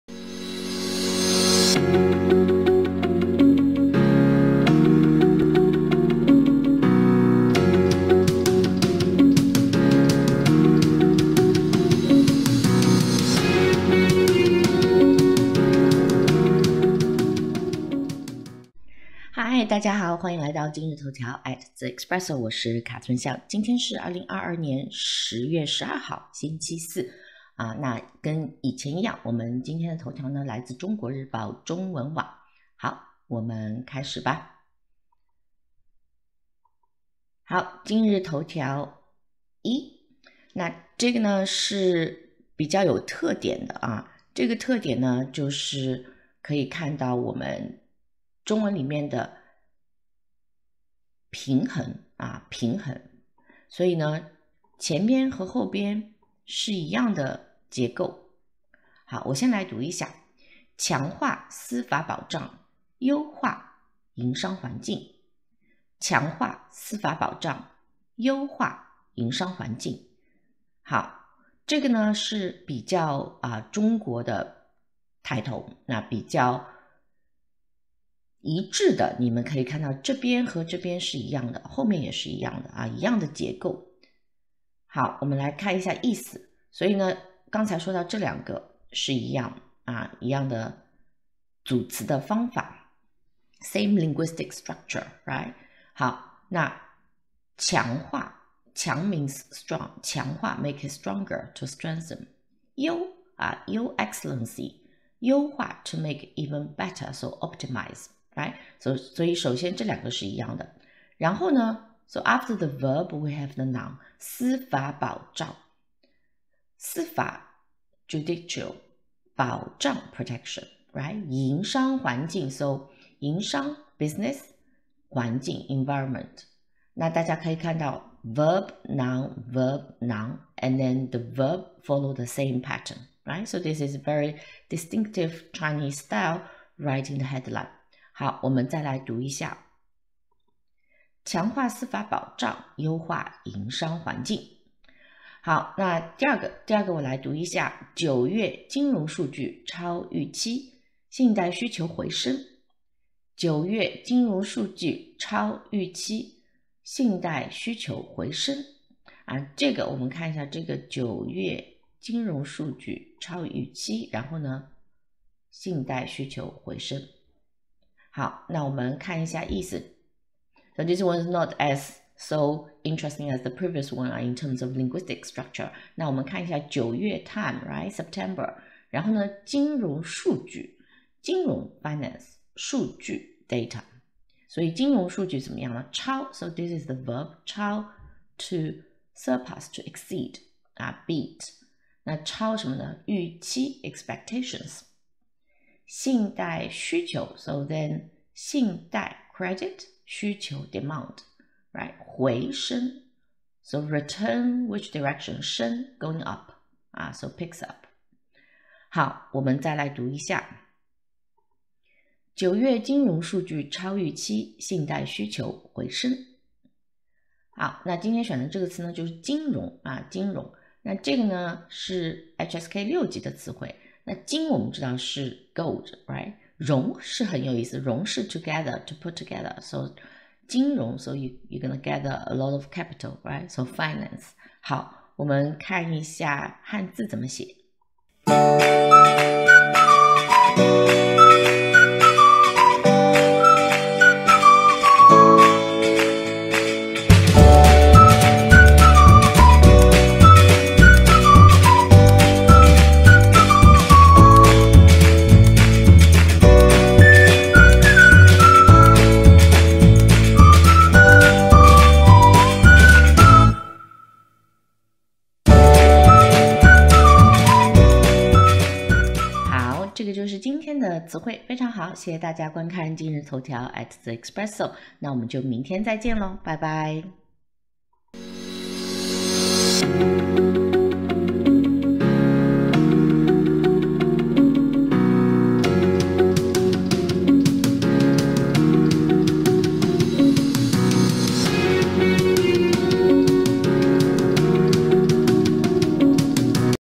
Learn Chinese from Daily Headline 今日头条 (Wednesday, 12 Oct 2022) - HSK 6
Welcome to Daily Headline 今日头条 at 字 espresso！

Today is Wednesday, 12 Oct 2022. Selected deadlines for you:

- China Daily 中文网：强化司法保障 优化营商环境
- China Daily 中文网：9月金融数据超预期 信贷需求回升

1. 强化 to strengthen
2. 司法 judicial
3. 保障 protection
4. 优化 optimise
5. 金融 financial, finance
6. 数据 data
7. 预期 expectation
8. 需求 demand

* Key characters:
金融 financial, finance  (HSK 6)

Example Sentences：
1. 但是，在金融世界里，你付多少钱就只能获得相等的回报。
Whereas in the financial world, you only get what you pay for.
2. 这也是为什么我觉得对于你们这些，立志从事那个行业的人来说，金融是个很有意思的领域。
So, that's why I think finance will be an interesting field for those of you who choose to go into it.
3. 因此，当今世界，美元的“一身二任”是国际金融风险之源。
Therefore the Dollar's "dual role" is the source of international financial risks.
4. 与此同时，就业、“三农”、节能减排、金融等方面也存在一些突出矛盾和问题。
Meanwhile, there are also acute problems concerning employment, agriculture, rural areas and farmers, energy conservation and pollution reduction and the financial sector.
5. 我国的实力使我们得以安然渡过这次金融危机。
The strength of our country bulwarked us through the financial crisis.

Follow more conversations and learn more Chinese:

- Purchase my textbook if you are a complete beginner